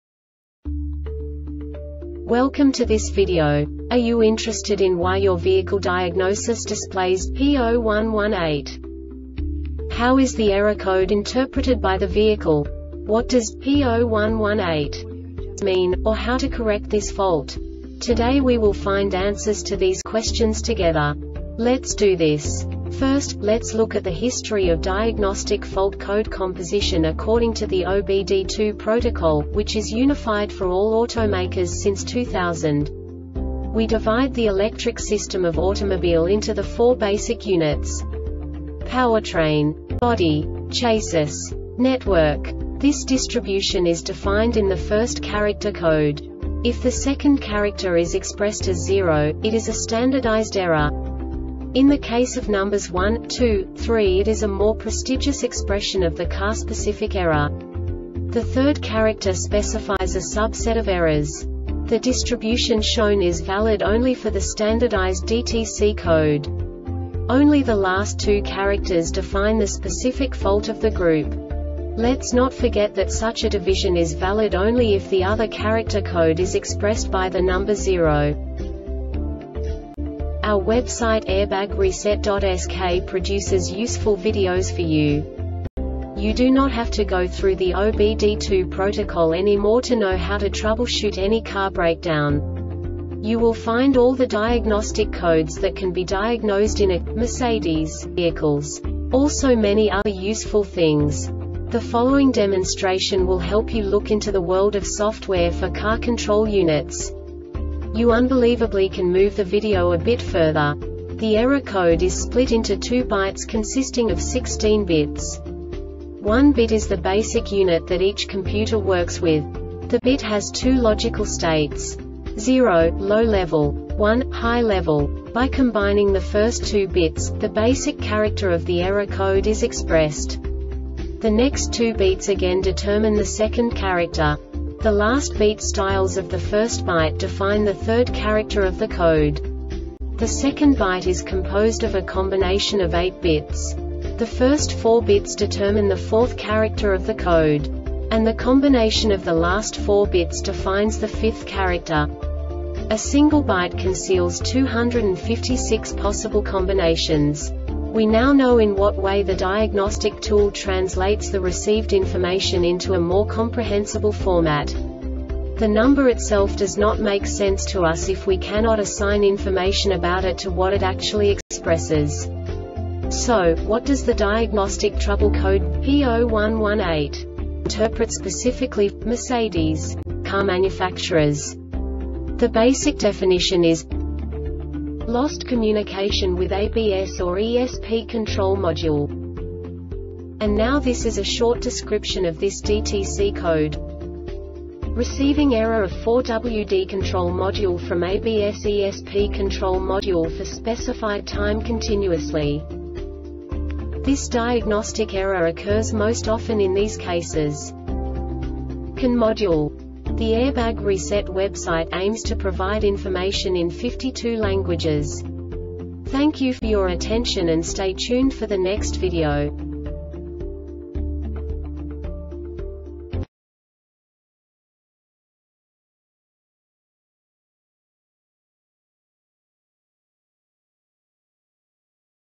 DTC Mercedes-Benz P0118 Short Explanation
Contents:
0:21 Basic DTC analysis according to OBD2 protocol standard.
1:48 Insight into programming
2:58 A diagnostically understandable description of the Diagnostic Trouble Code
3:05 Basic error definition

Make
Mercedes Benz
Code
P0118
Definition
Lost Communication With ABS  or  ESP® Control Module
Description
Receiving error of 4WD control module from ABS  or  ESP control module for specified time continuously
Cause
CAN Module